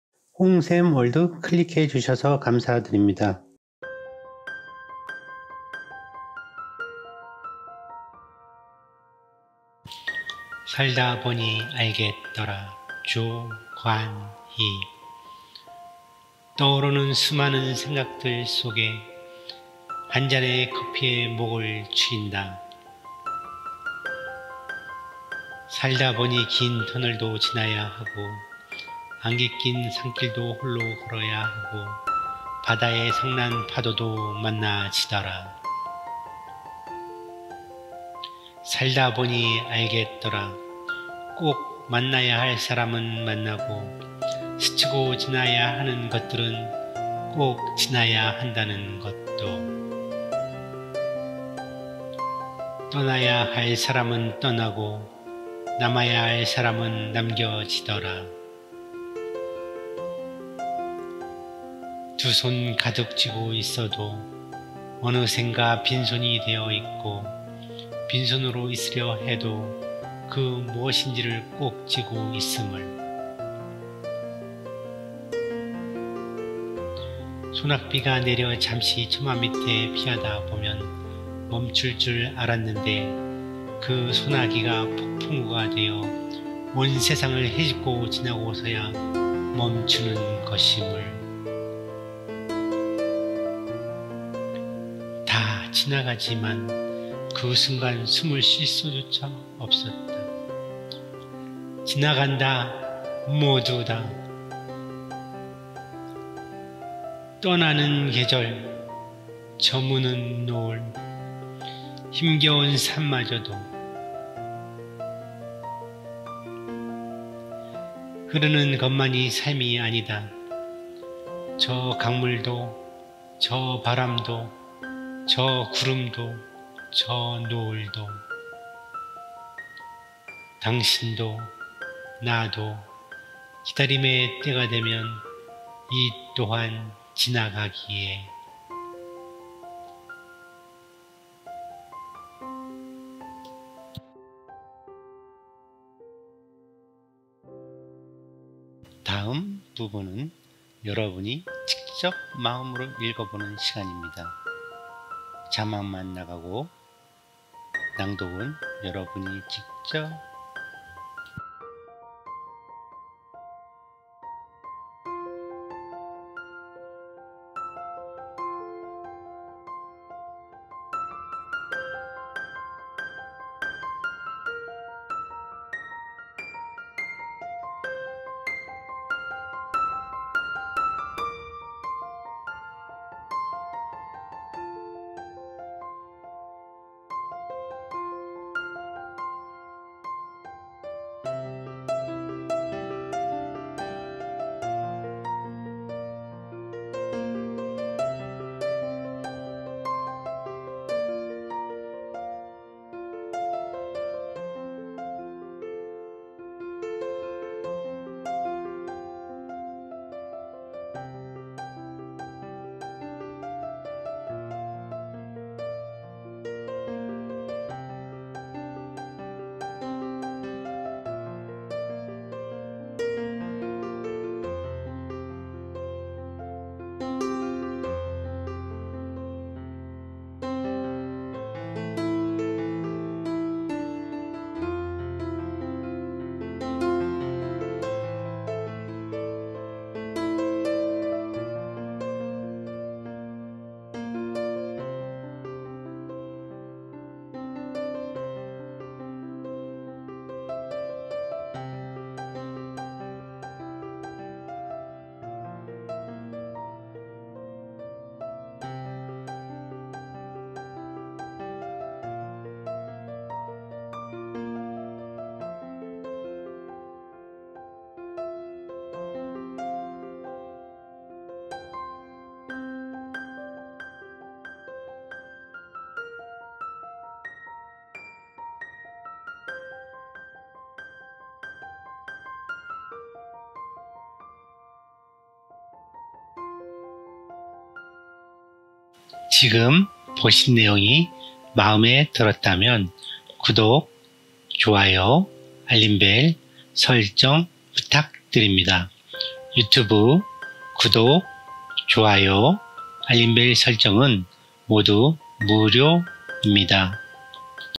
좋은시, + 살다 보니 알겠더라-조관희, 고난과 역경 극복 시, 인생의 운명을 바꾸는시
좋은시
고난과 역경 극복 시
인생의 운명을 바꾸는시

+ 살다 보니 알겠더라-조관희

살다 보니 긴 터널도 지나야 하고
안개 낀 산길도 홀로 걸어야 하고
바다의 성난 파도도 만나지더라

살다 보니 알겠더라
꼭 만나야 할 사람은 만나고
스치고 지나야 하는 것들은
꼭 지나야 한다는 것도

떠나야 할 사람은 떠나고
남아야 할 사람은 남겨지더라.

다 지나가지만
그 순간 숨을 쉴 수조차 없었다
지나간다 모두 다

흐르는 것만이 삶이 아니다
저 강물도, 저 바람도
저 구름도, 저 노을도